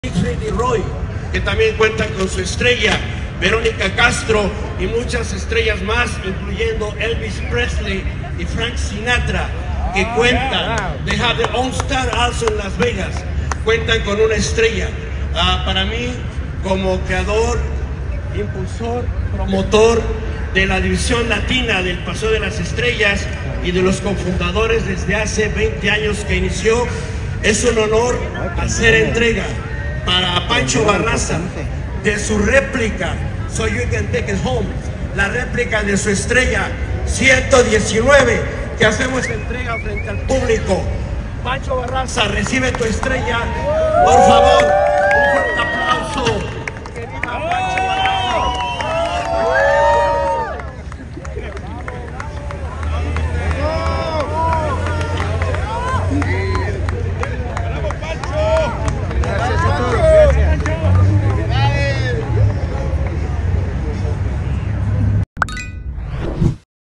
PANCHO BARRAZA YA TIENE SU ESTRELLA ⭐️ EN LAS VEGAS, el cantante la develo con toda su familia
Pancho Barraza se sigue posicionando en el regional mexicano, el cantante develo la noche de ayer su estrella en el Paseo de la Fama de Las Vegas acompañado de grandes amigos y de toda su familia.

SÍGUEME